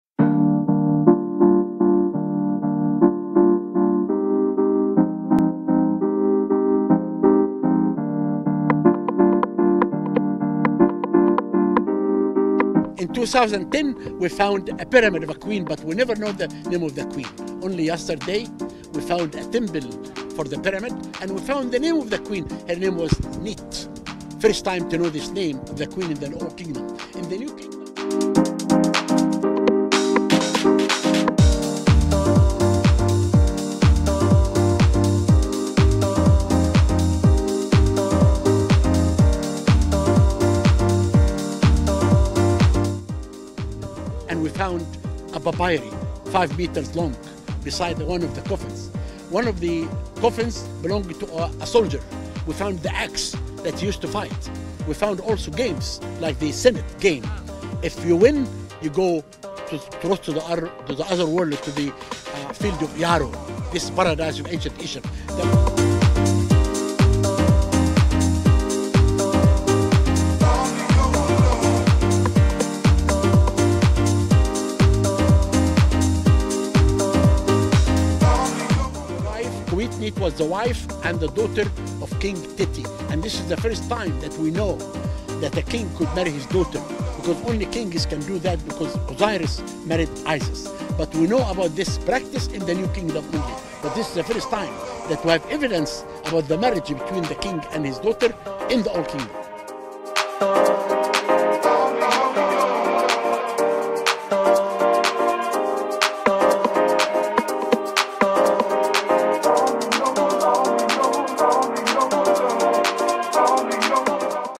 مصر تكشف عن كنوز عمرها ثلاثة آلاف عام ! 🇪🇬
مصر تكشف عن كنوز جديدة مكتشفة في مقبرة سقارة ... اكتشفوا العجائب التي يبلغ عمرها أكثر من 3000 عام ! 🇪🇬